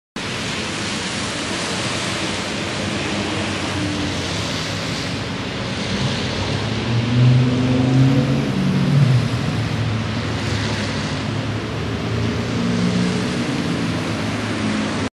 2013 02 08 Robin Hooding Keene x021
Linda and Jane dodge multiple merry men the day before the big blizzard